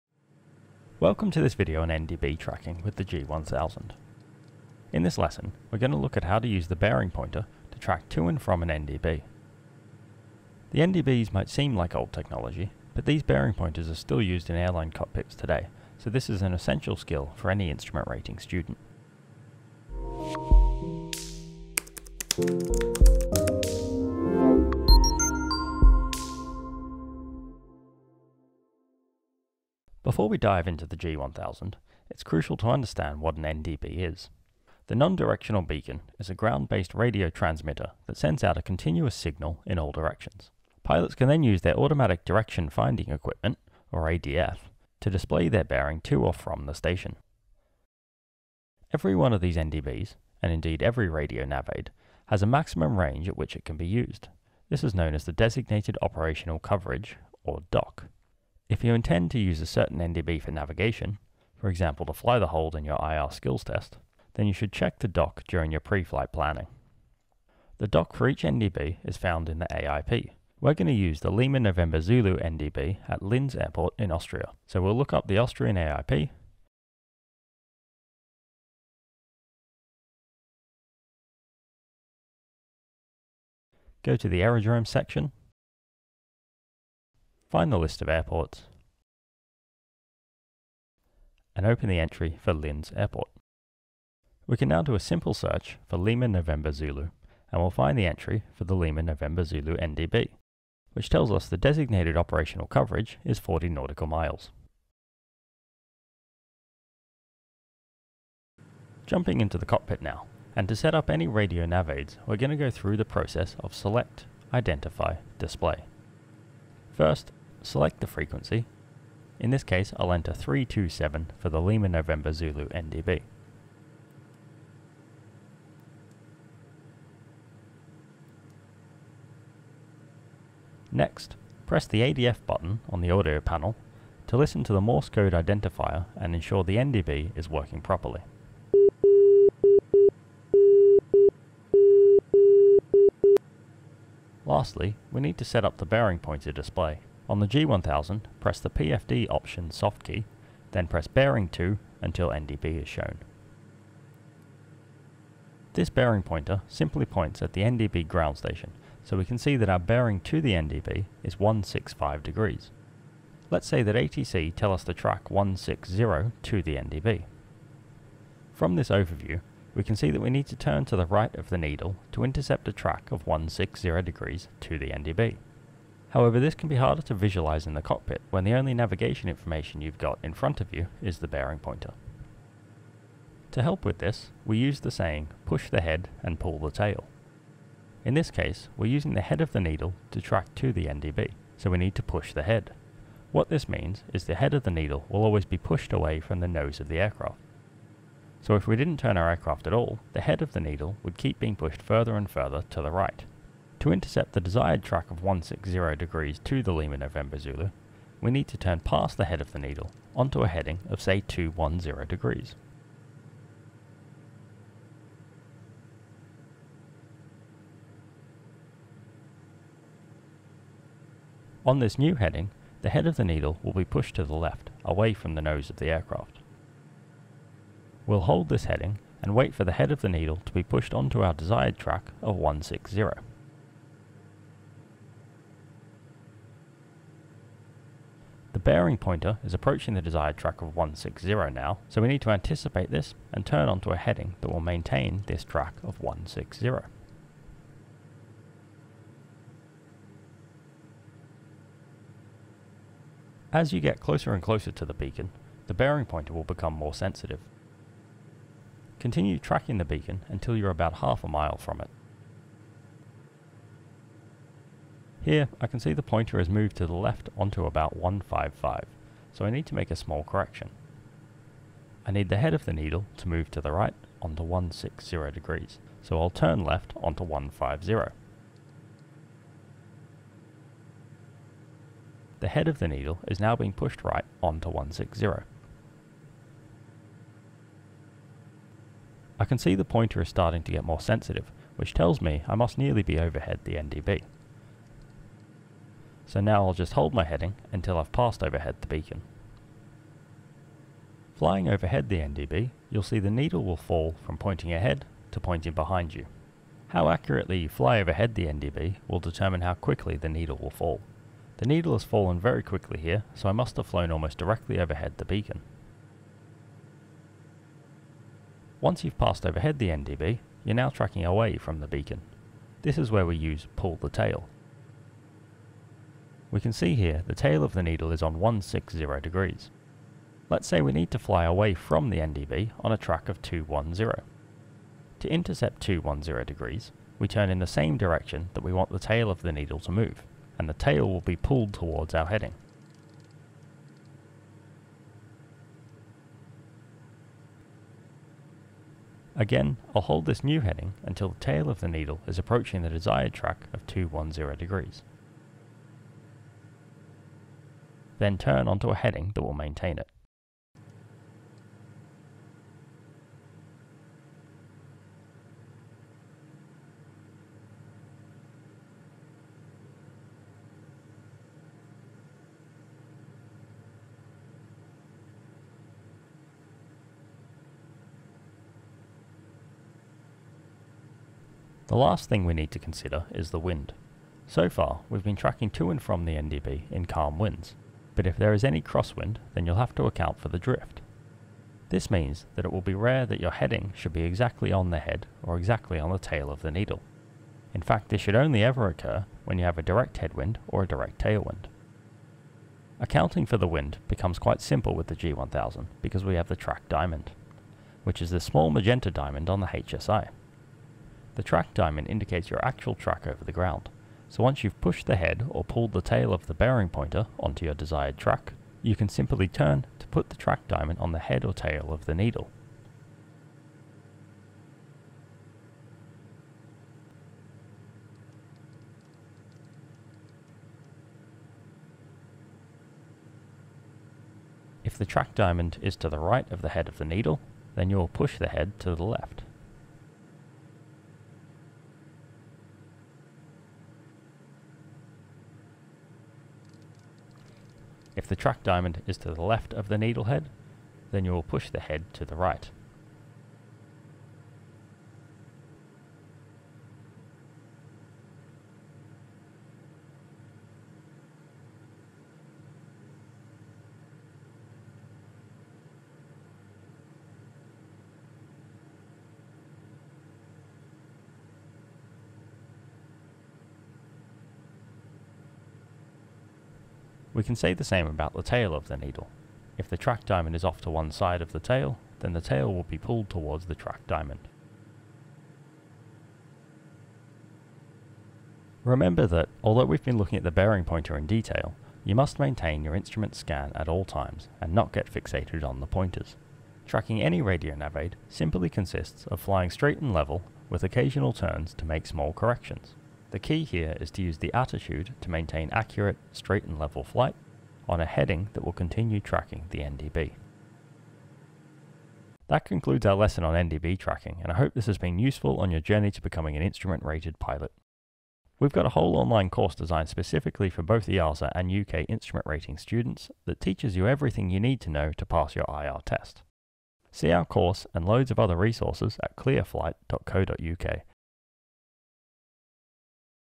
NDB Tracking | EASA & UK Instrument Rating
In this video, we look at how to use the Automatic Direction Finding (ADF) equipment to intercept and track radials to & from a Non-Directional Beacon (NDB).

For everything you need to pass your EASA or UK Instrument Rating, see our online course